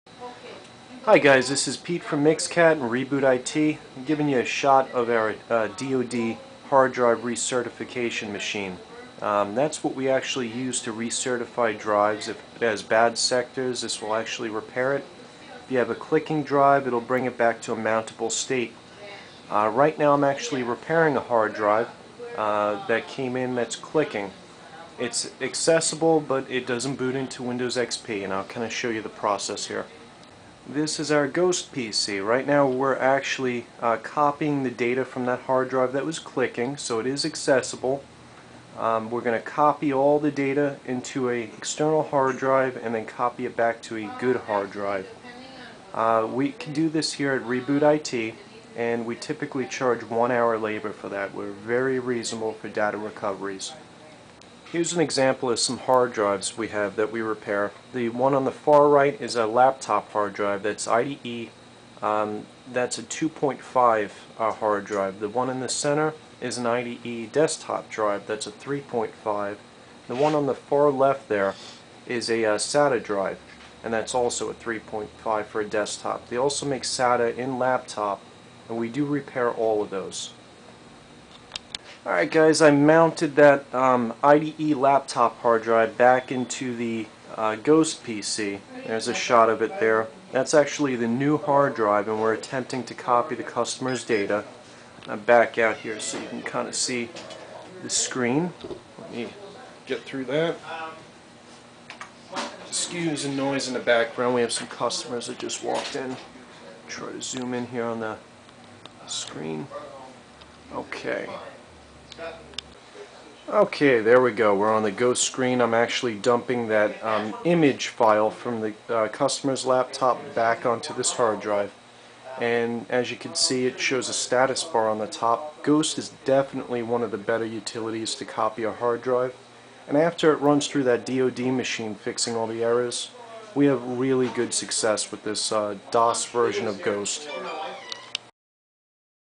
How to recover data from IDE SCSI Sata hard drives
This is how we recover data from hard drives.
It dont matter if they are from a laptop notebook, server or desktop. IED, SCSI or Sata.  He is how we do it.